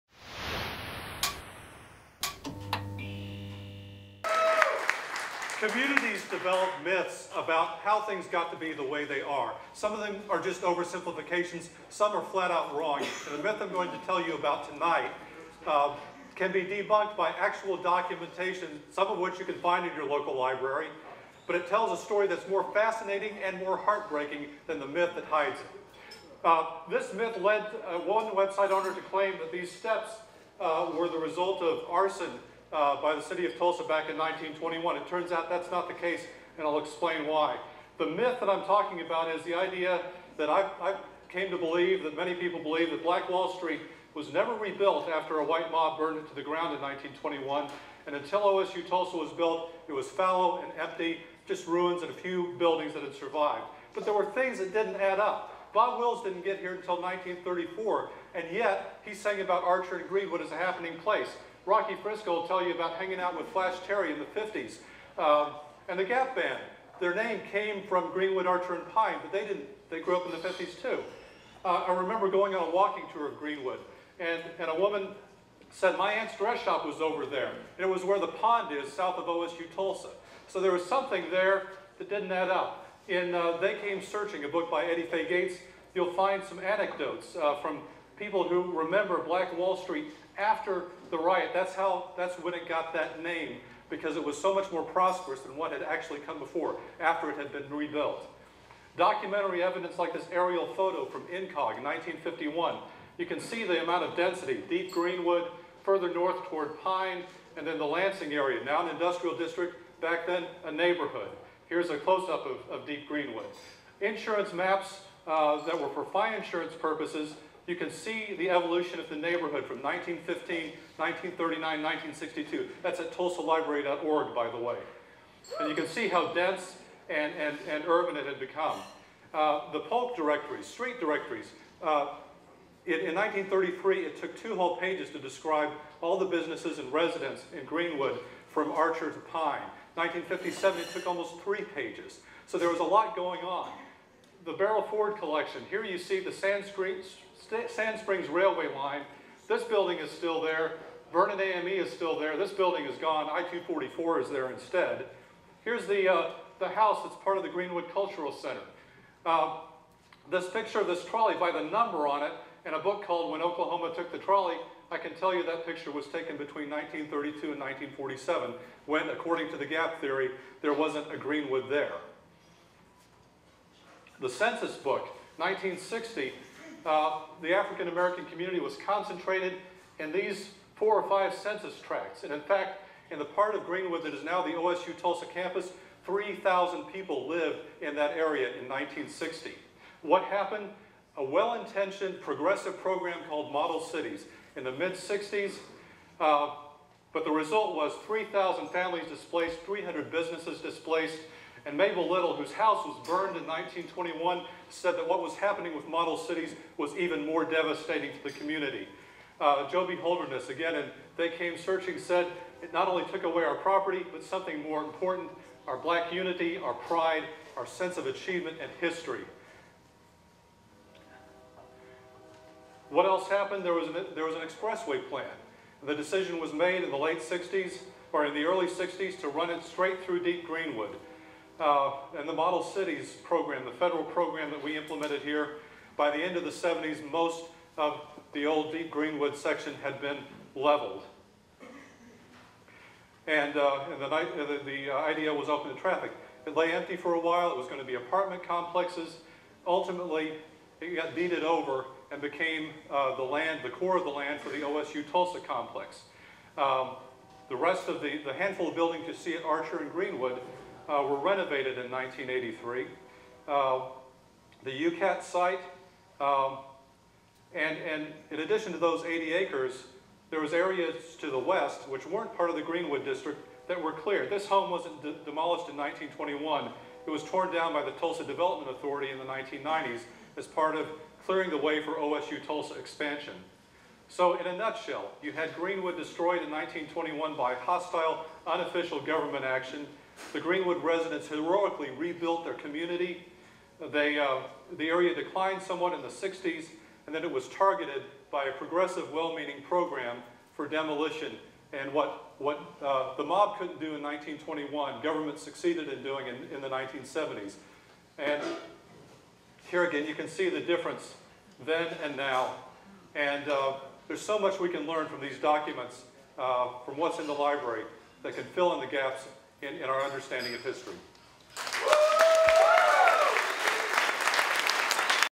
The Greenwood Gap, Michael Bates - Ignite Tulsa
The Greenwood Gap: Mythbusting pop history with contemporary documents by Michael Bates - Ignite Tulsa